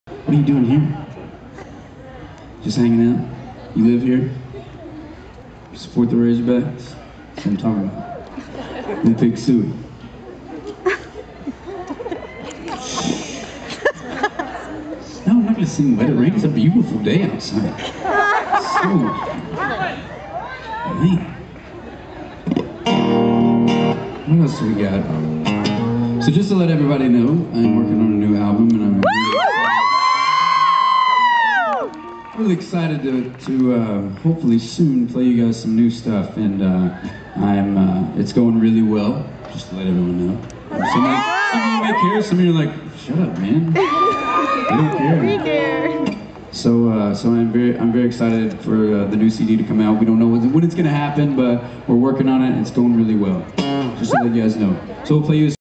Banter: Kris talks about new album - Clearwater, FL 3/13/11
Kris finds a razorback fan in the crowd and talks about the new album he's working on! :)